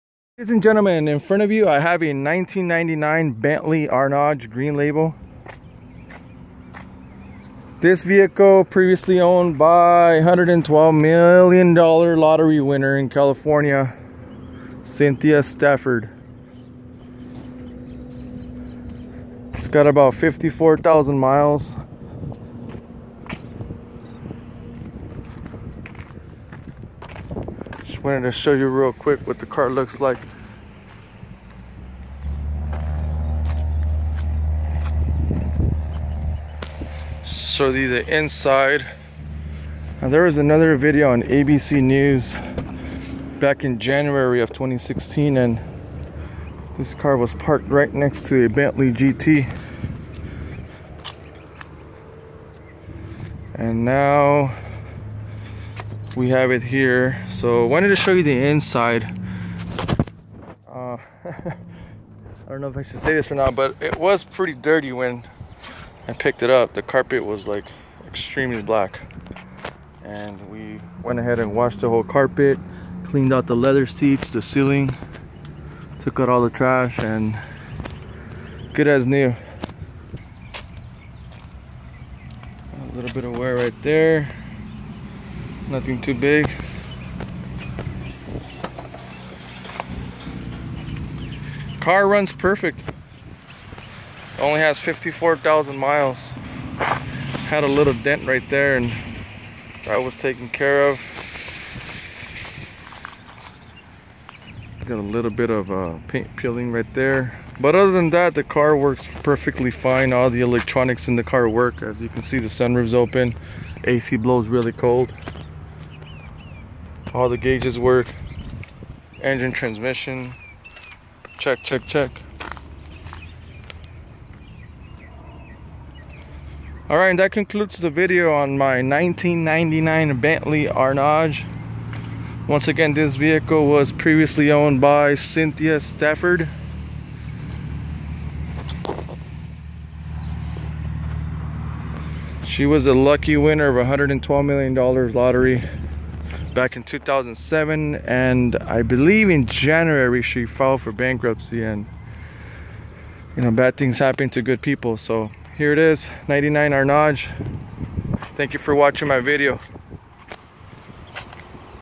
Cynthia Stafford LOTTERY WINNER - inside her BENTLEY ARNAGE
Bad things happen to good people... here is a peek inside her baby blue Arnage.